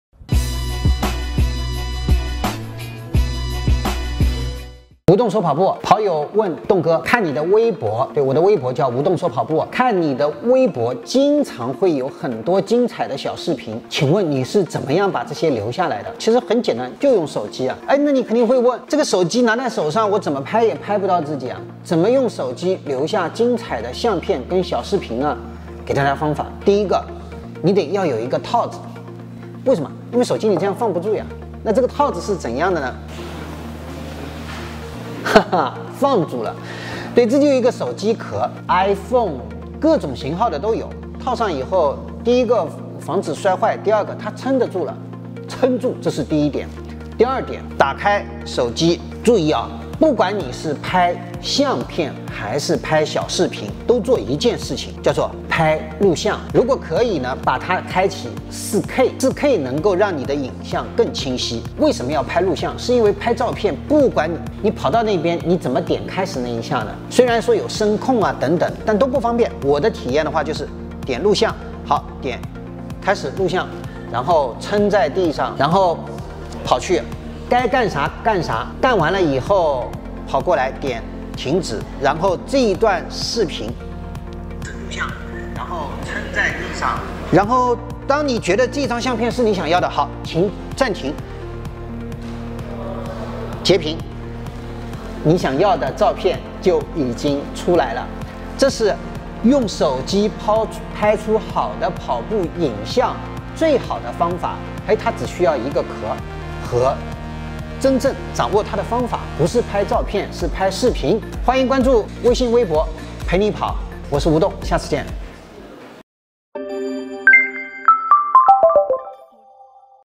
How to use a mobile phone to take a run 吴栋说跑步：如何用手机拍摄跑步照片和视频
吴栋说跑步：如何用手机拍摄跑步照片和视频 245